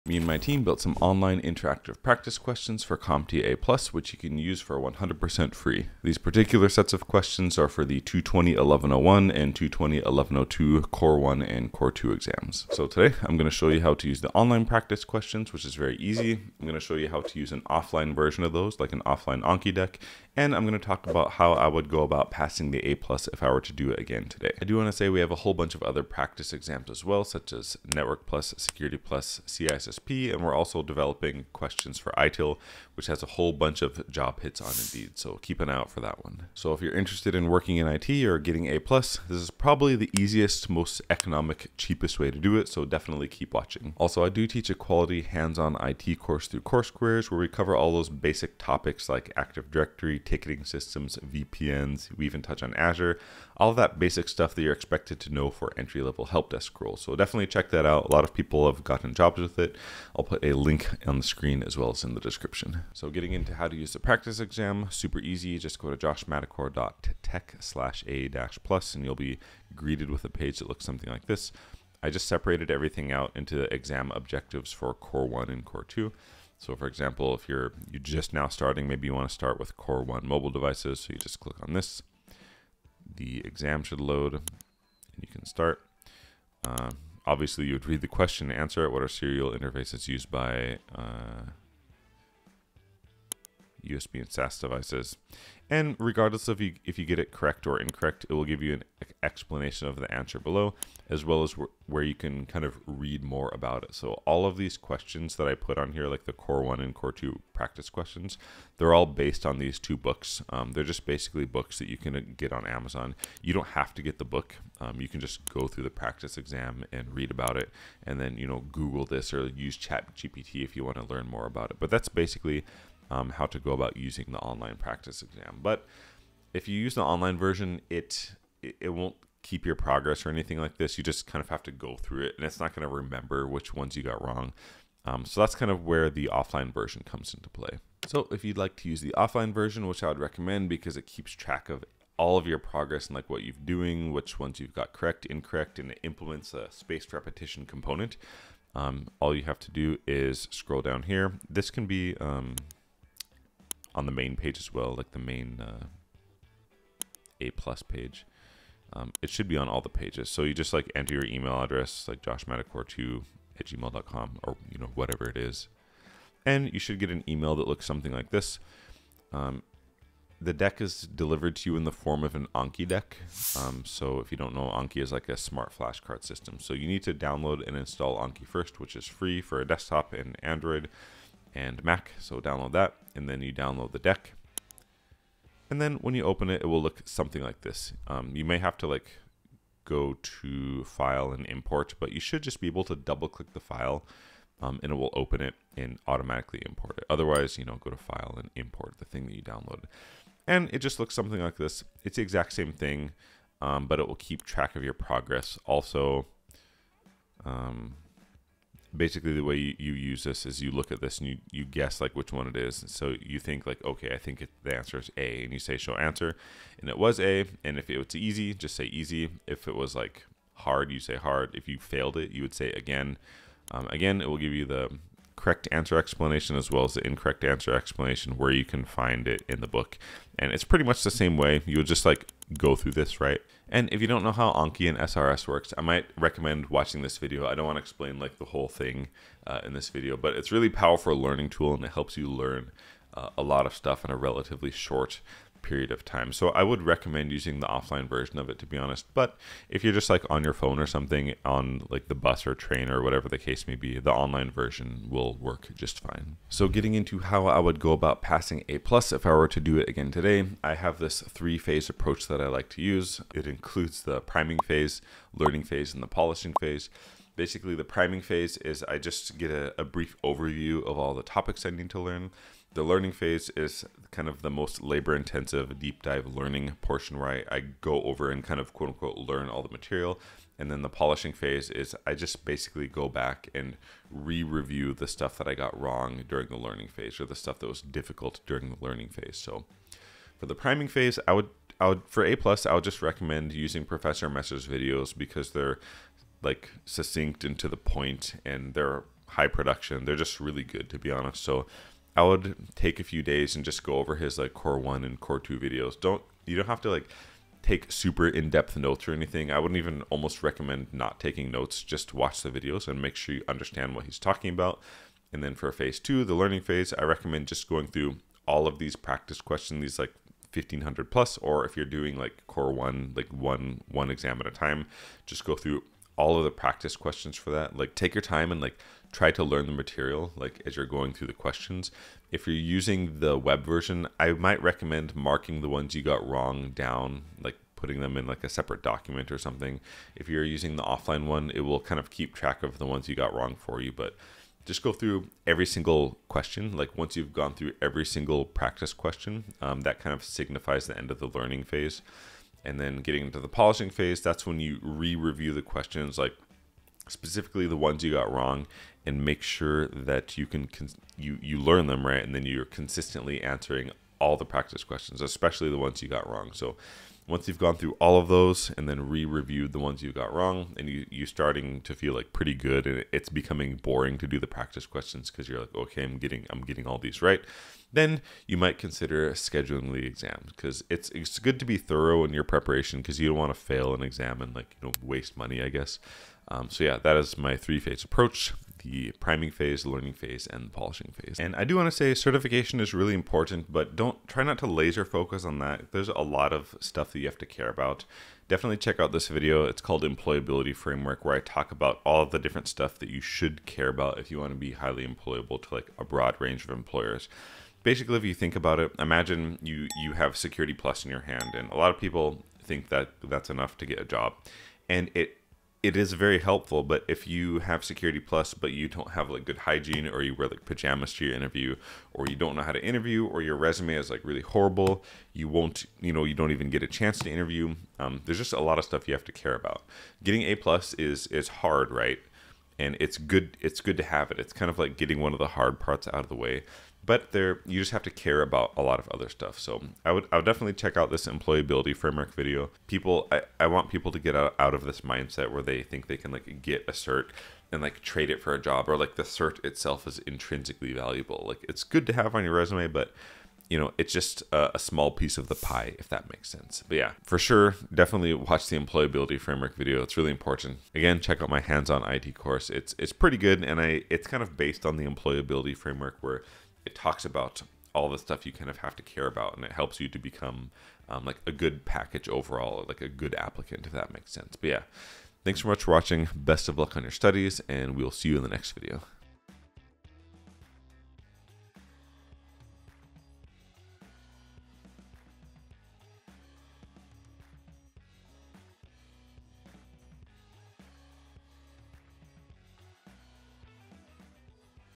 How to PASS CompTIA A+ in 2024 (Over 1500+ Practice Questions)
🖥️ New IT Career in under 3 Months with my Hands-On Course 🖥

In this video, I delved into how to pass CompTIA A+ in 20204 and give away an extensive collection of over 1500+ new practice questions for the CompTIA A+ certification, offering a comprehensive resource for IT professionals who are earnestly preparing to ace their certification exam and enhance their knowledge in the field.

00:00 Intro
00:12 What To Expect?
01:02 How to Use Practice Exams?
01:11 How to Use Online Exams?
02:17 Offline Version’s Benefits/How to Use Offline Exams?
05:16 How I would Study to Pass A+ today?
07:41 Conclusion

Welcome to our video, where we talk about the CompTIA A+ certification. This video is a must-watch if you're getting ready for your IT certification test. We've gathered more than 1500 practice questions to help you study. These questions cover all the topics you need to know for the exam. We've made sure to include questions on hardware, networking, security, and troubleshooting. This is your one-stop resource for everything CompTIA A+.

Studying for the CompTIA A+ exam can be overwhelming. There's so much to learn, from computer parts to how networks work. But don't worry, we've got your back. Our practice questions are designed to make you feel like you're taking the real test. This means you can practice in a way that's close to the actual exam environment. Plus, answering these questions helps you remember the material better.

Remember, passing the CompTIA A+ exam opens many doors in the IT field. It's the first step many people take towards a successful IT career. This certification shows employers that you have what it takes to work with computer systems and networks. It's recognized worldwide, so it can help you find a job or get a better one wherever you go.

Take notes if you need to, and don't rush. You can pause and rewatch any part that you find tricky. We're here to help you succeed, and we believe that with the right preparation, you can pass the CompTIA A+ exam.